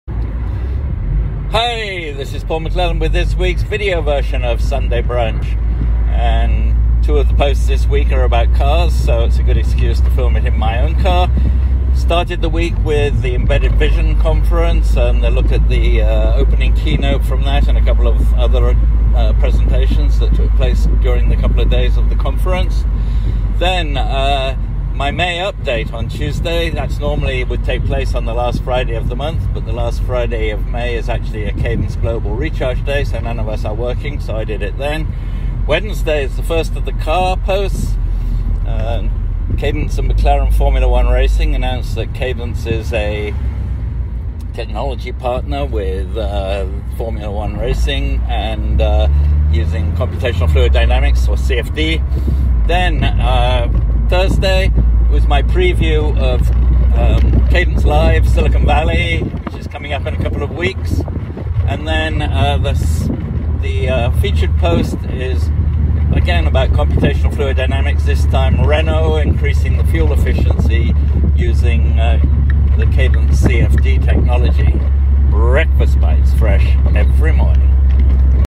Sunday Brunch 29th May
Paul McLellan serves up blogs for the week with the latest cutting-edge technology available at Cadence Design Systems.

Made in my Mini Cooper Convertible

Monday: Embedded Vision Summit 2022

Tuesday: May Update: ACM Digital Library, Open RAN Security, Framework Laptop Upgrade, Malcolm Penn, and More

Wednesday: Cadence Shifts into High Gear with the McLaren Formula 1 Team

Thursday: CadenceLIVE Silicon Valley 2022 Preview

Friday: Vacation

Featured Post: Renault Reduces Fuel Consumption by Optimizing Engine Parts with CFD